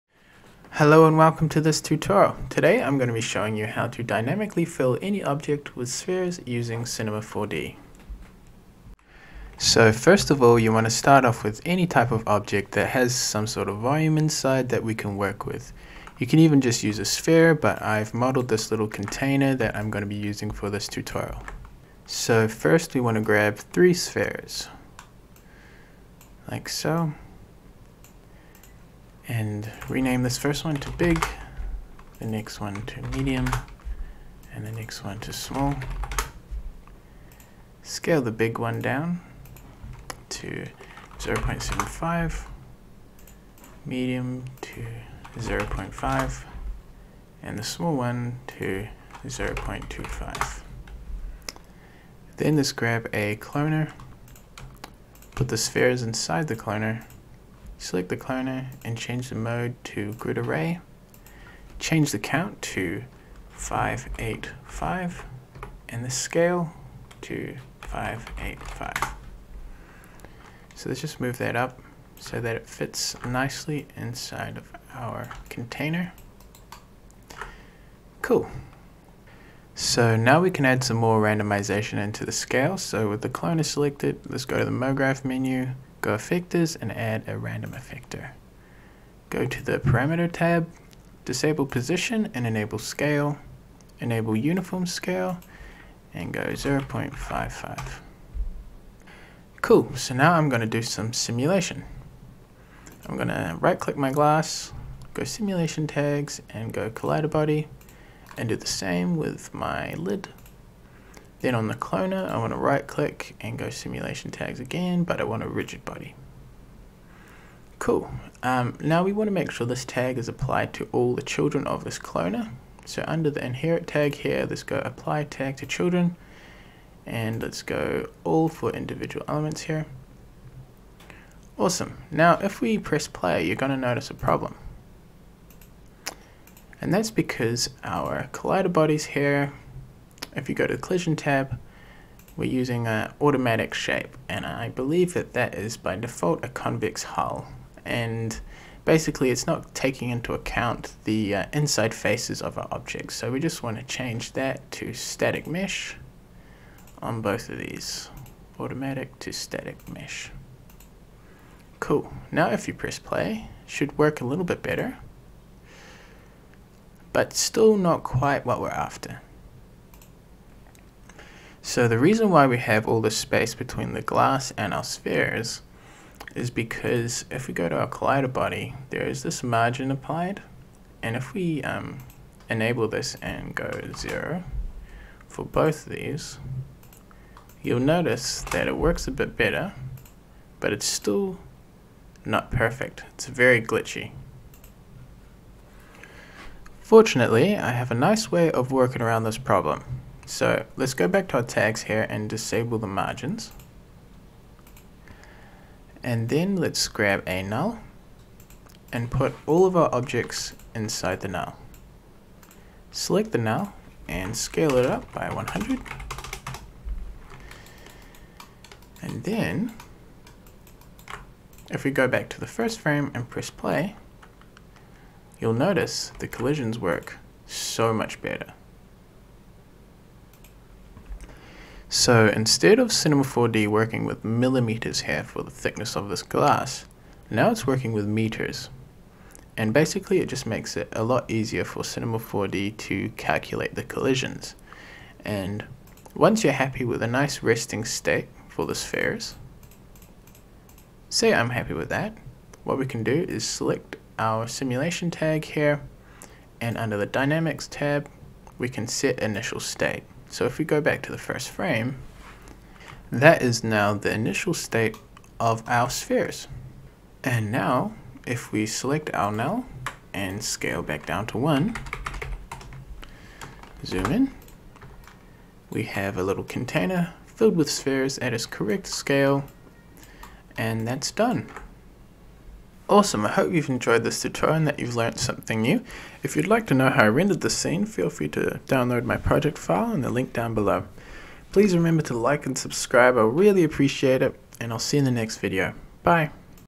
Filling Objects With Spheres in Cinema 4D - Tutorial
A Tutorial that demonstrates how to dynamically fill an object with spheres using Cinema 4D R20.